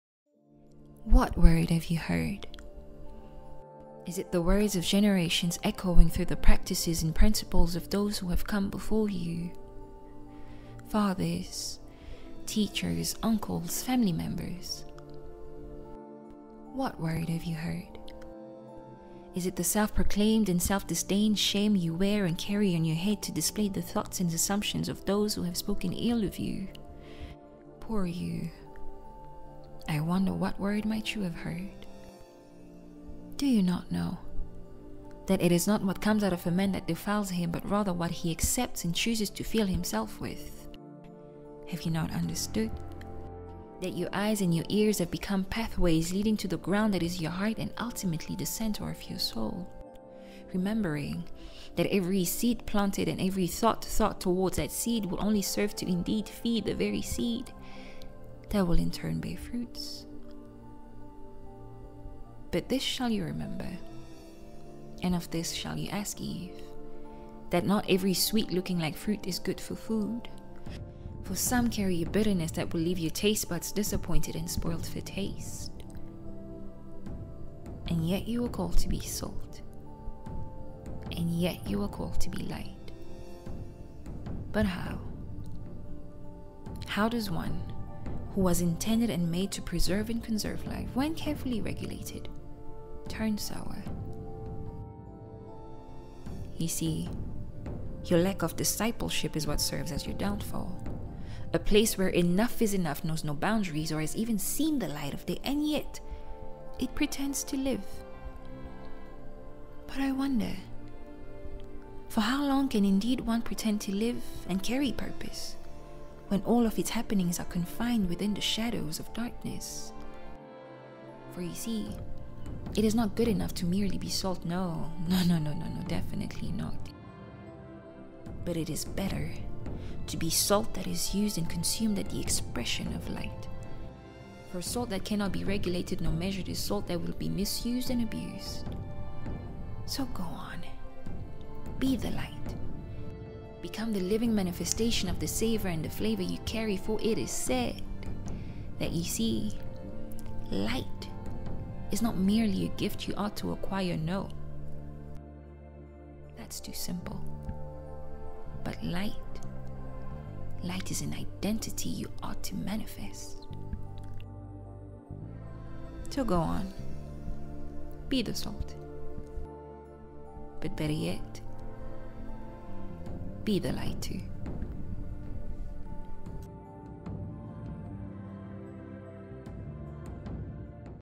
Full Highlights SHIFT 2023 (SEL ET LUMIÈRE) | Ps Guelord Lukama
Poem written and narrated by Candide Luabeya